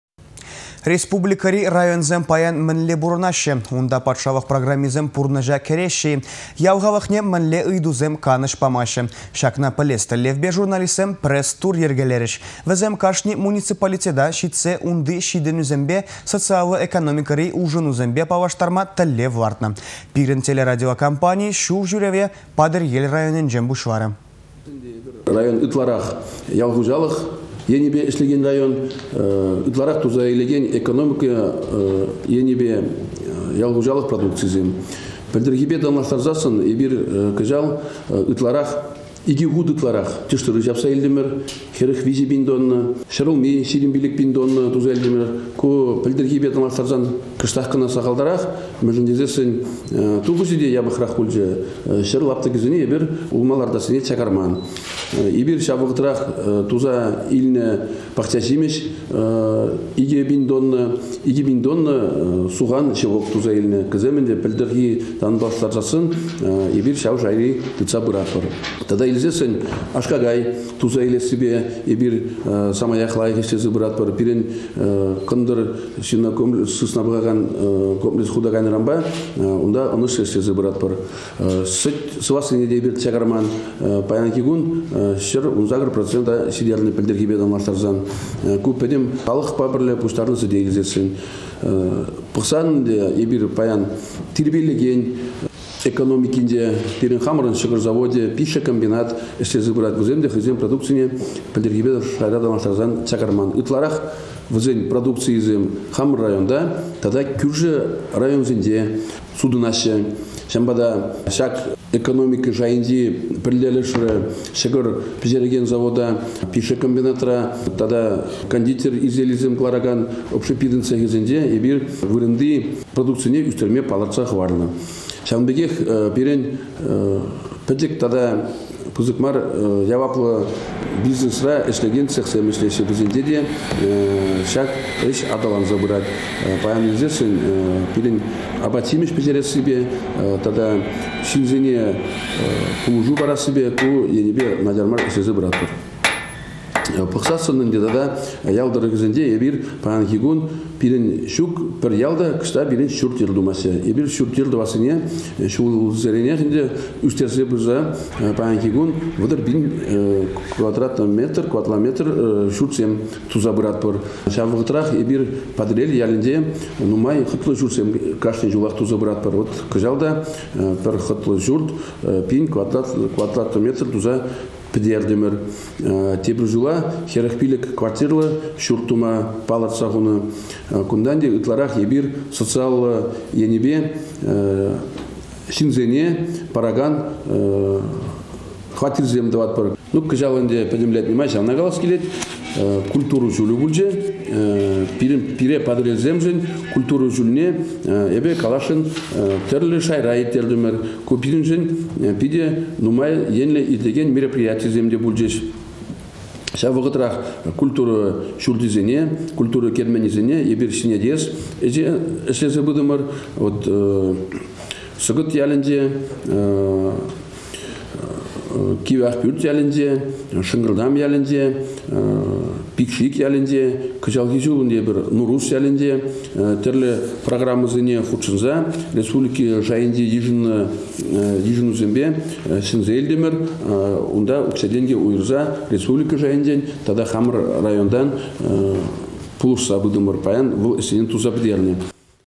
Патăрьелĕнче пурăнакансем паянхи кăтартусемпе лăпланасшăн мар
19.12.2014
Республикăри районсем паян мĕнле пурăнаççĕ, унта патшалăх программисем пурнăçа кĕреççĕ-и? Ял халăхне мĕнле ыйтусем канăç памаççĕ? Çакна пĕлес тĕллевпе журналистсем пресс-тур йĕркелерĕç. Вĕсем кашни муниципалитета çитсе унти çитĕнÿсемпе, социаллă экономикăри улшăнусемпе паллаштарма тĕллев лартнă. Пирĕн телерадиокомпани çул çÿреве Патăрьел районĕнчен пуçларĕ.